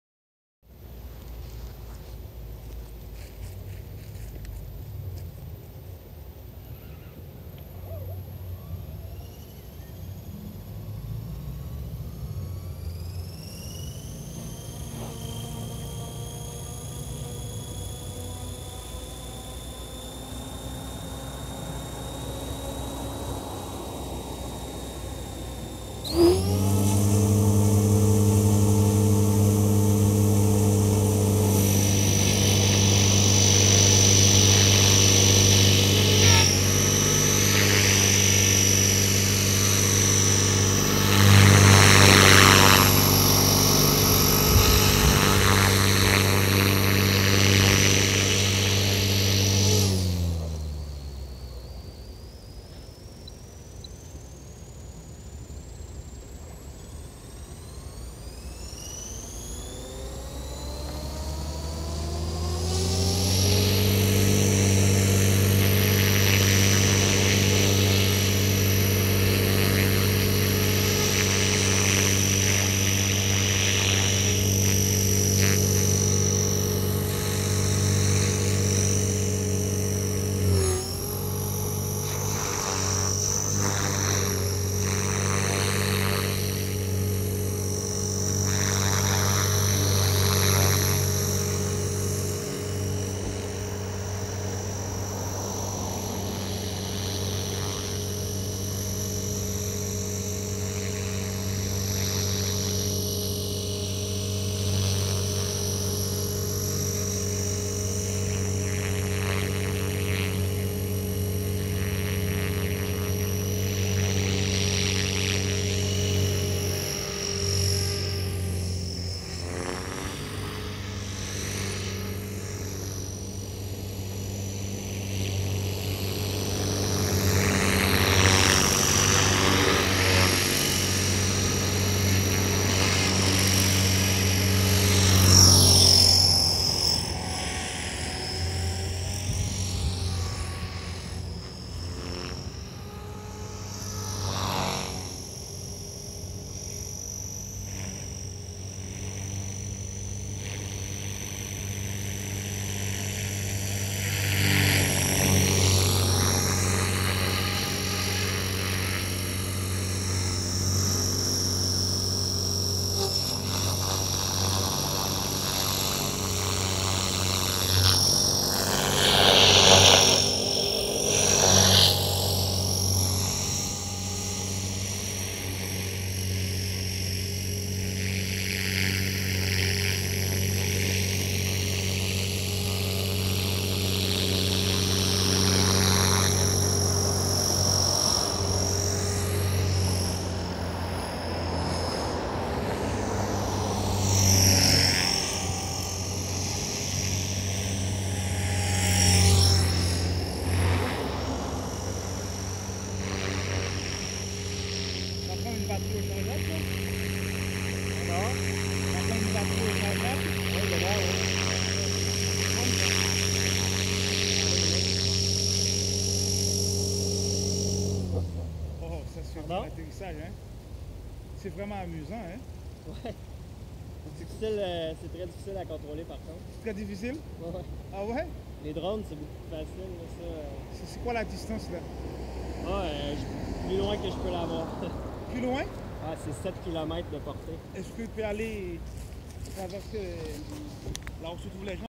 500 size RC heli, new setup maiden flight. 2016
First flight with m'y new setup!
Sorry for thé pour point of view... nobody to hold the camera!

-Heli: Quick Japan EP8 v2 EX (better than t-rex 500 but company is out of business)
-FBL upgrade: HK500 with ZYX S2 controller
-5000mah 6cells battery (twice as powerful as m'y original setup!)
-Transmitter: good Old Spektrum DX7 with single stick modification.